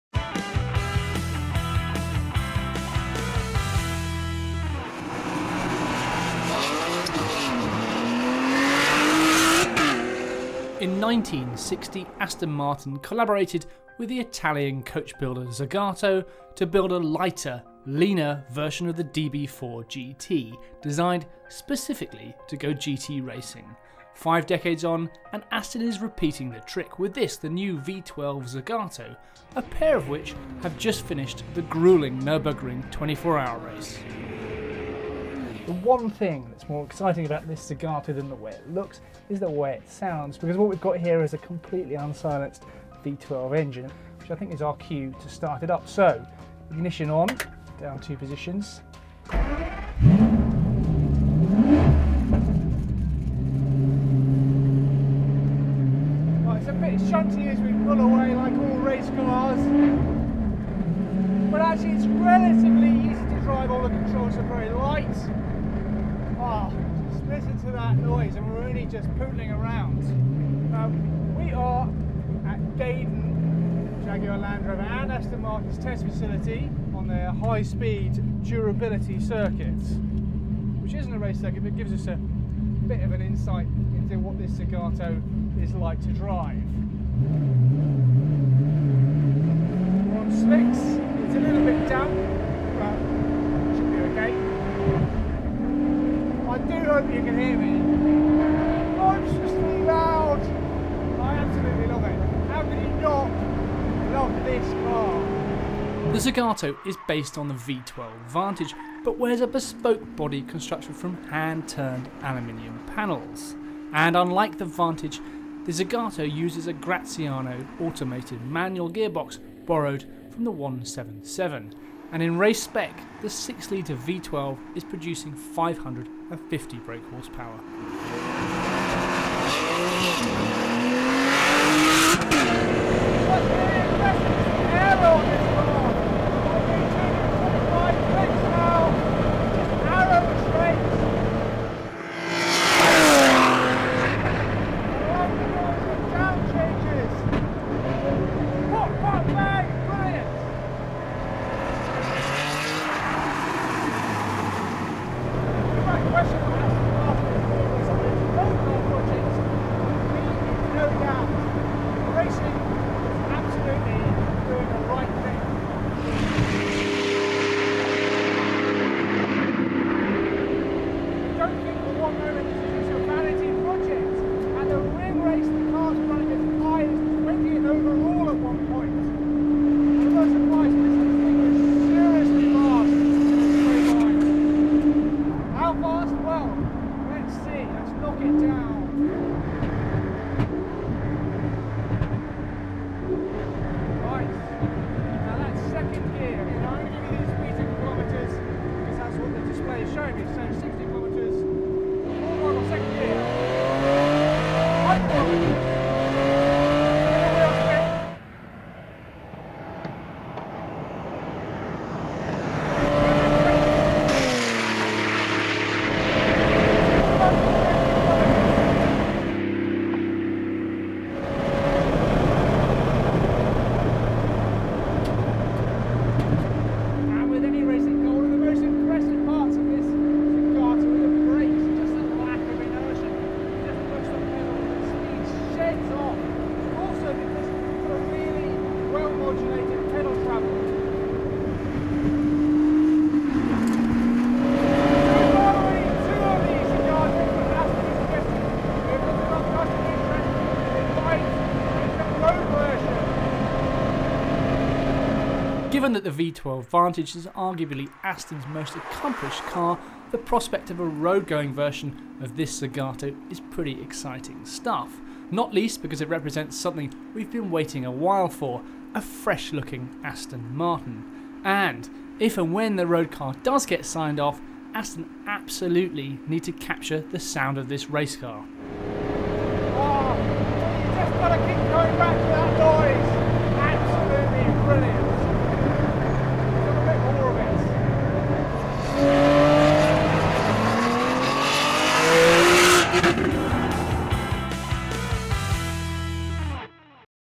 Aston Martin Zagato race car video review
Aston Martin's new Zagato race car looks fantastic. But the real highlight here comes courtesy of the car's completely unsilenced 6.0-litre V12 engine. It produces a mammoth 550bhp, which is transferred to the tarmac via an automated manual gearbox borrowed from the Aston One-77. So what is this stripped out racer like to drive? Here, Jamie Corstorphine finds out at Gaydon's high-speed test facility.